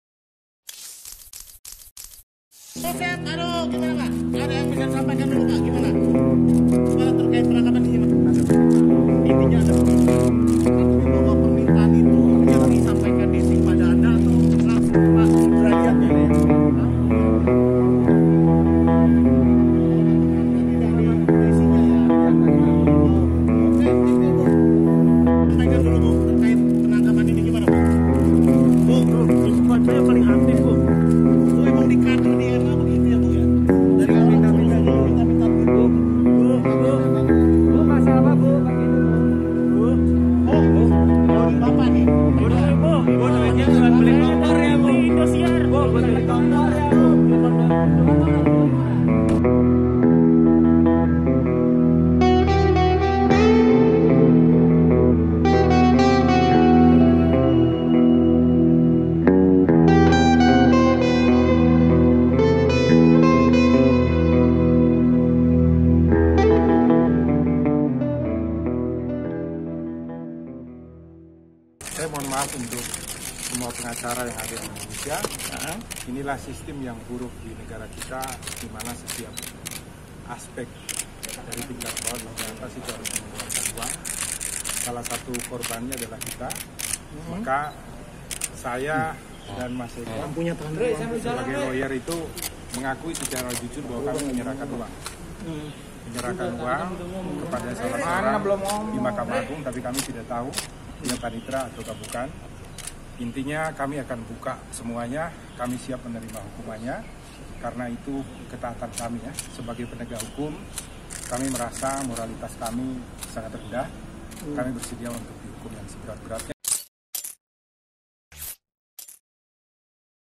Ditetapkan Tersangka Kasus Dugaan Suap di MA, Pengacara Ini Mengaku sebagai Korban
Komisi Pemberantasan Korupsi (KPK) menetapkan Pengacara Yosep Parera sebagai tersangka dalam kasus dugaan suap penanganan perkara di Mahkamah Agung. Dia mengklaim menjadi korban dari sebuah sistem buruk yang ada di Indonesia karena menurutnya sistem itu memaksa pengurusan perkara harus dibarengi dengan pemberian uang.

Yosep mengaku salah setelah memberikan suap dan dia juga memohon maaf kepada seluruh pengacara di Indonesia karena perbuatannya. Yosep siap dihukum dengan vonis berat dan dia juga yakin permasalahan ini bisa membuatnya tidak memberikan suap kepada Pejabat Negara setelah masa penahanan selesai.